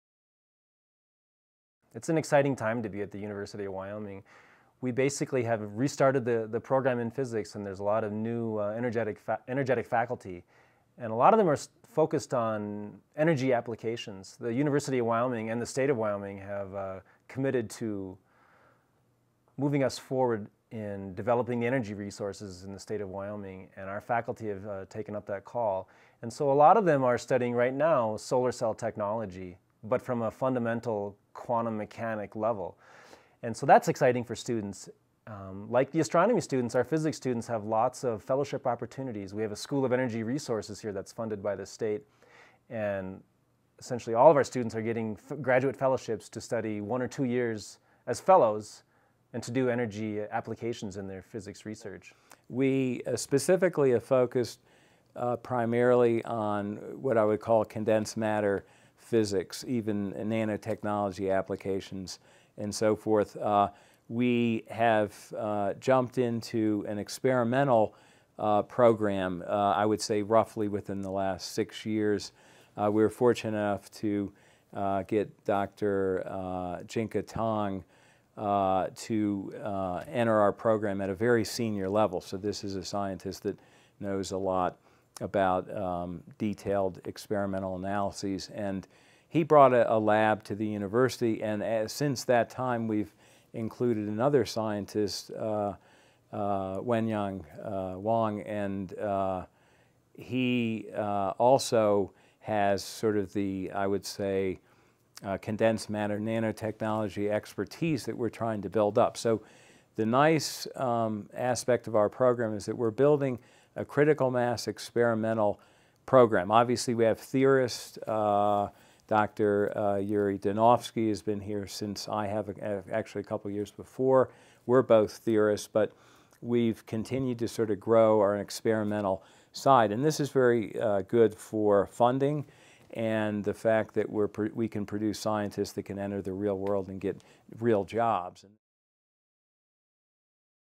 Overview of the Department of Physics and Astronomy at the University of Wyoming
An overview of the Physics & Astronomy Department at UW. for more information.